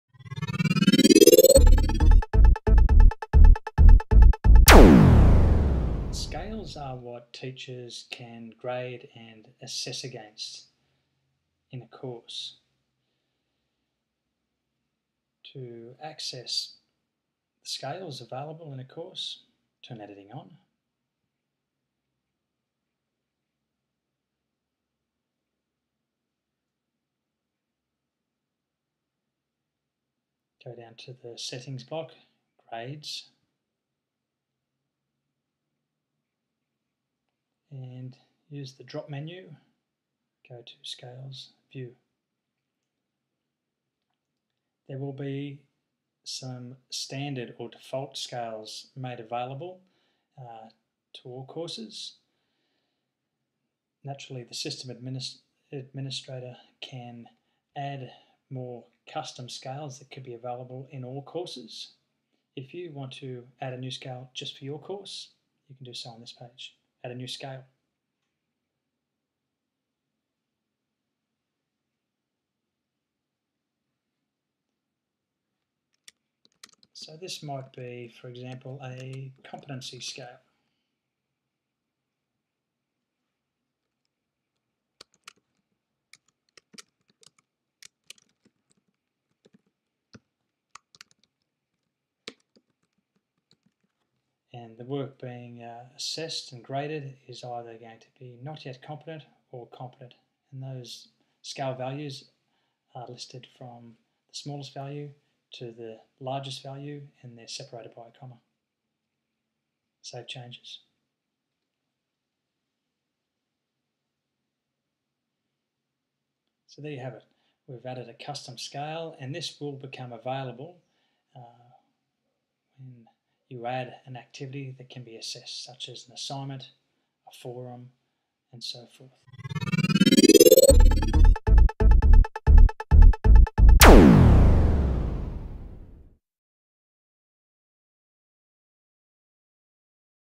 Moodle - Scales - 036
To learn more about what a Learning Management System can do for you,

This tutorial demonstrates how to use scales for the Moodle learning management system.